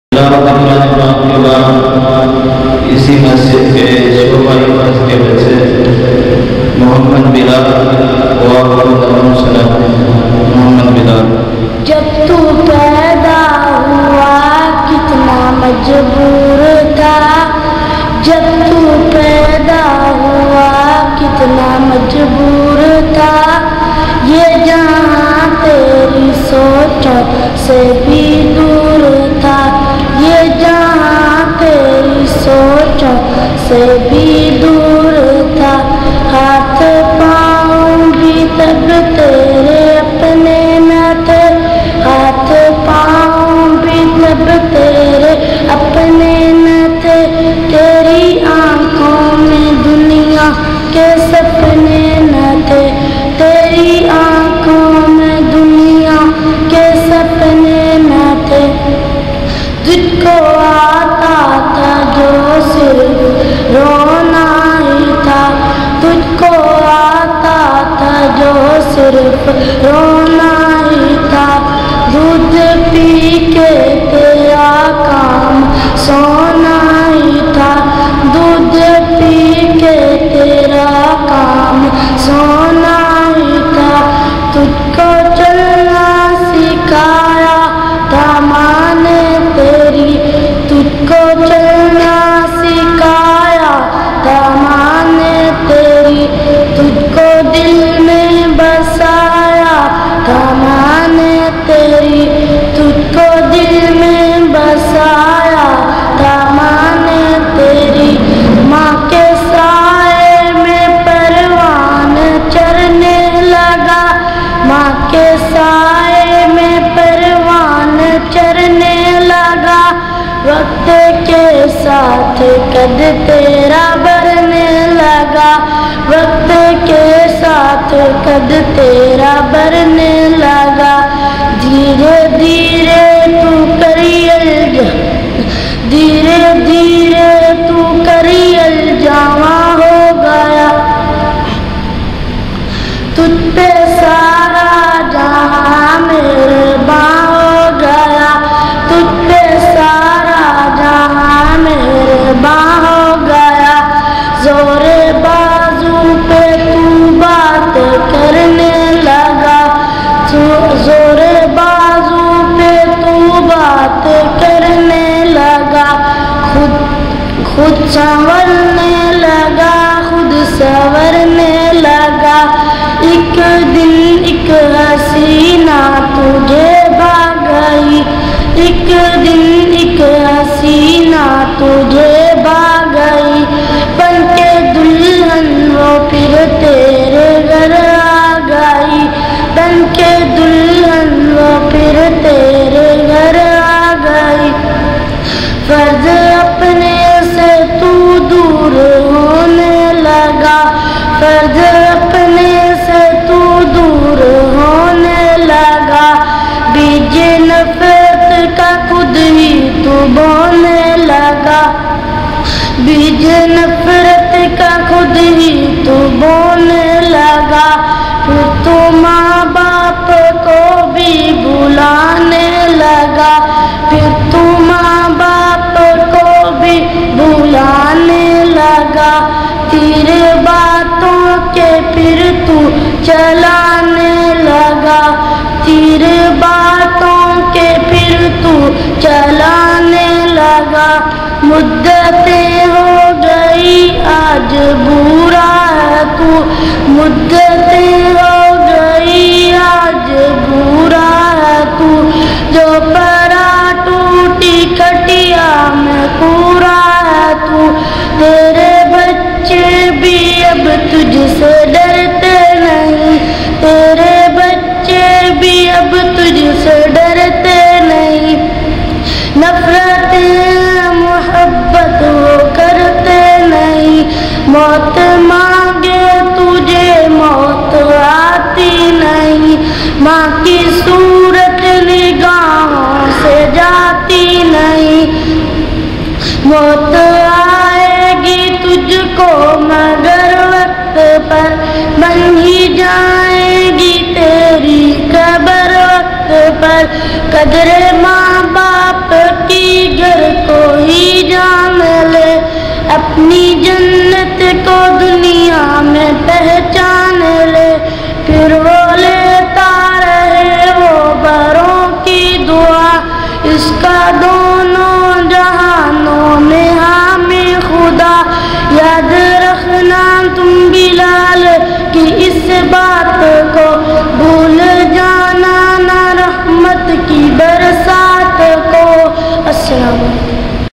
Jab Tu Paida Hua Kitna Majboor Tha Viral Video || Beautiful Urdu Poem Ever about parents .
Jab Tu Paida Hua Kitna Majboor Tha Viral Video || Beautiful Urdu Poem Ever about parents . SAYYAD TAYYAB UR RAHMAN SHAH ZAIDI AUR TMAM ULAMA E AHLY HADEES KI VIDEOS MOJOOD HAIN